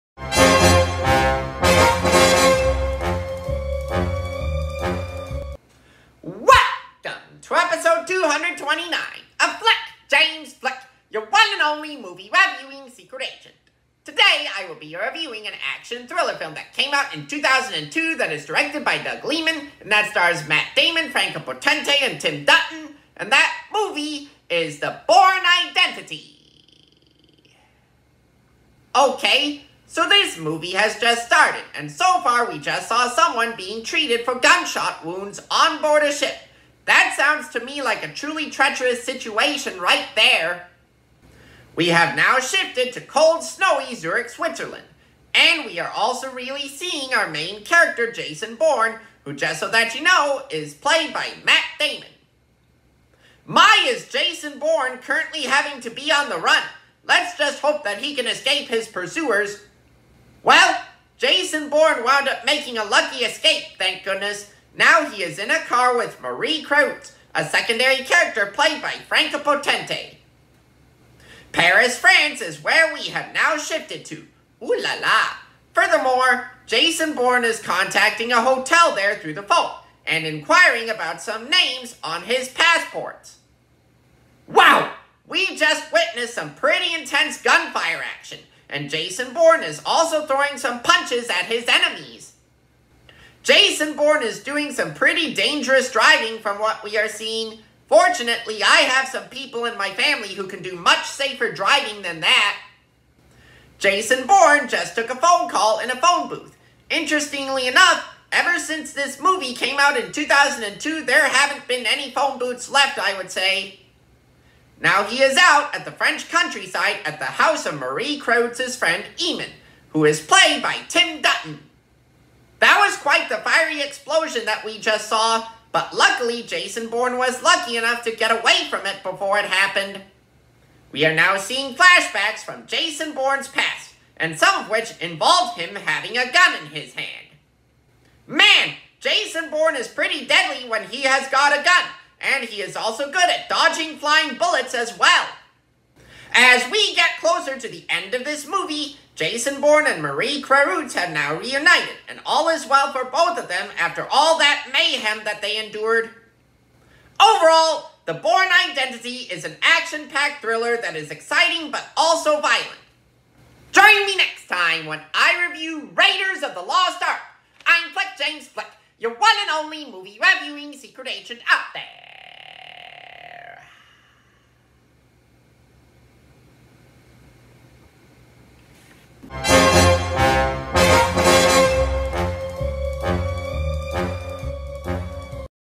Episode 229: The Bourne Identity
In Episode 229, I give my thoughts of and reactions to The Bourne Identity, which is an action thriller film that is directed by Doug Liman and that stars Matt Damon as Jason Bourne, Franka Potente as Marie Kreutz, and Tim Dutton as Eamon.